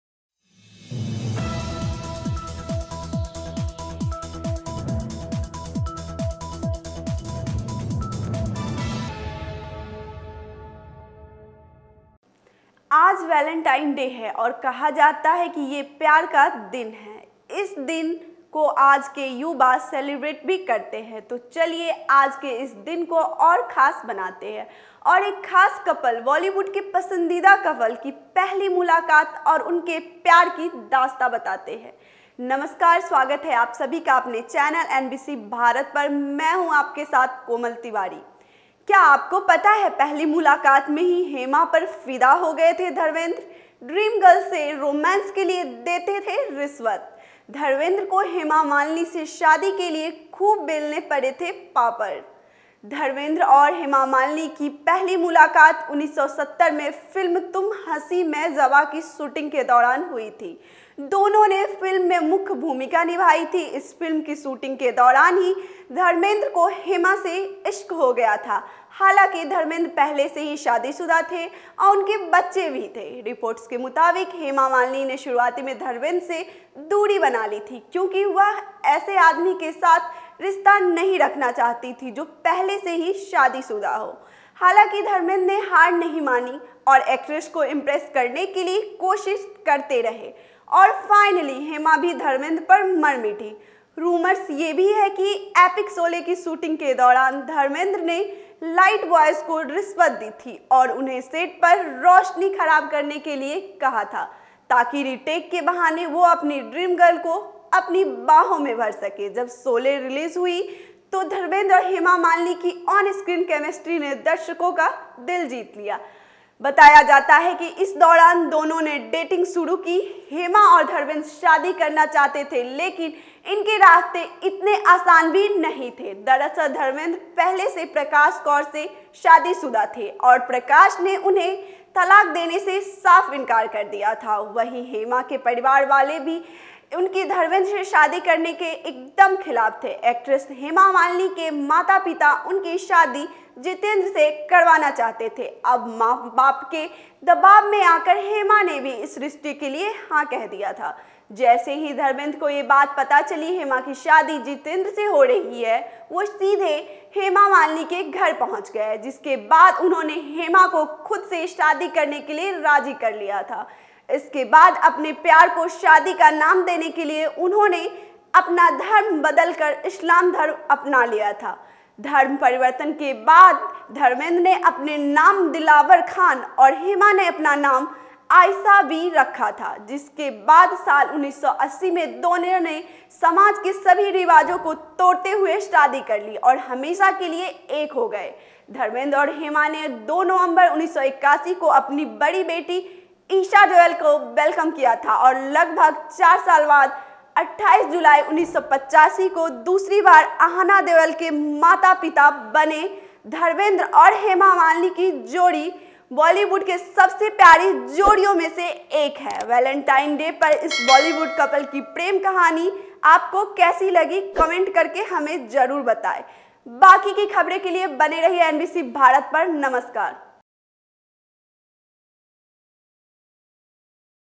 Valentine Day Special: वैलेंटाइन पर Bollywood के पसंदीदा जोड़ी Dharmendra और Hema Malini की प्रेम कथा
Valentine Day Special: वैलेंटाइन पर Bollywood के पसंदीदा जोड़ी Dharmendra और Hema Malini की प्रेम कथा NBC Bjarat

क्या आपको पता है...पहली मुलाकात में ही हेमा पर फिदा हो गए थे धर्मेंद्र, 'ड्रीम गर्ल' से रोमांस के लिए देते थे रिश्वत,धर्मेंद्र को हेमा मालिनी से शादी के लिए खूब बेलने पड़े थे पापड़. धर्मेंद्र और हेमा मालिनी की पहली मुलाकात 1970 में फिल्म ‘तुम हसीं मैं जवान’ की शूटिंग के दौरान हुई थी... दोनों ने फिल्म में मुख्य भूमिका निभाई थी... इस फिल्म की शूटिंग के दौरान ही धर्मेंद्र को हेमा से इश्क हो गया था... हालांकि, धर्मेंद्र पहले से ही शादी-शुदा थे और उनके बच्चे भी थे...रिपोर्ट्स के मुताबिक, हेमा मालिनी ने शुरुआत में धर्मेंद्र से दूरी बना ली थी.. क्योंकि.. वह ऐसे आदमी के साथ रिश्ता नहीं रखना चाहती थीं जो पहले से ही शादीशुदा हो... हालांकि, धर्मेंद्र ने हार नहीं मानी और एक्ट्रेस को इम्प्रेस करने के लिए कोशिश करते रहे...और फाइनली हेमा भी धर्मेंद्र पर मर मिटी...रूमर्स ये भी हैं कि एपिक शोले की शूटिंग के दौरान, धर्मेंद्र ने.. लाइट बॉयज़ को रिश्वत दी थी.. और उन्हें सेट पर रोशनी खराब करने के लिए कहा था ताकि.. रीटेक के बहाने वे अपनी ड्रीम गर्ल को अपनी बाहों में भर सकें...जब शोले रिलीज़ हुई, तो धर्मेंद्र और हेमा मालिनी की ऑन-स्क्रीन केमिस्ट्री ने दर्शकों का दिल जीत लिया... बताया जाता है कि इसी दौरान दोनों ने डेटिंग शुरू की. हेमा और धर्मेंद्र शादी करना चाहते थे लेकिन इनके रास्ते इतने आसान भी नहीं थे... दरअसल धर्मेंद्र पहले से प्रकाश कौर से शादीशुदा थे और प्रकाश ने उन्हें तलाक देने से साफ इंकार कर दिया था... वहीं हेमा के परिवार वाले भी उनकी धर्मेंद्र से शादी करने के एकदम खिलाफ थे.. एक्ट्रेस हेमा मालिनी के माता-पिता उनकी शादी जितेंद्र से करवाना चाहते थे...अब मां-बाप के दबाव में आकर हेमा ने भी इस रिश्ते के लिए हां कह दिया था. जैसे ही धर्मेंद्र को ये बात पता चली हेमा की शादी जितेंद्र से हो रही है.. वे सीधा हेमा मालिनी के घर पहुंच गए... जिसके बाद उन्होंने हेमा को खुद से शादी करने के लिए राजी कर लिया था... इसके बाद अपने प्यार को शादी का नाम देने के लिए.. उन्होंने अपना धर्म बदलकर इस्लाम धर्म अपना लिया था... धर्म परिवर्तन के बाद धर्मेंद्र ने अपना नाम दिलावर खान और हेमा ने अपना नाम आयशा बी रखा था... जिसके बाद साल 1980 में दोनों से समाज के सभी रिवाजों को तोड़ते हुए शादी कर ली और हमेशा के लिए एक हो गए...धर्मेंद्र और हेमा ने 2 नवंबर, 1981 को अपनी बड़ी बेटी ईशा देओल का वेलकम किया था और लगभग चार साल बाद, 28 जुलाई, 1985 को, दूसरी बार अहाना देओल के माता-पिता बने...धर्मेंद्र और हेमा मालिनी की जोड़ी बॉलीवुड की सबसे प्यारी जोड़ियों में से एक है...वैलेंटाइन डे पर इस bollywood कपल की प्रेम कहानी.. आपको कैसी लगी... कमेंट करके हमें जरुर बतांए...बाकी की खबरे के लिए बने रहिए NBC भारत नमस्कार..

Our Team:-
• Video Editor: Varun Baghel
• Cameraman: Varun Baghel
• Anchor/Producer: Komal Tiwari
• Follow Anchor on Instagram /Twitter :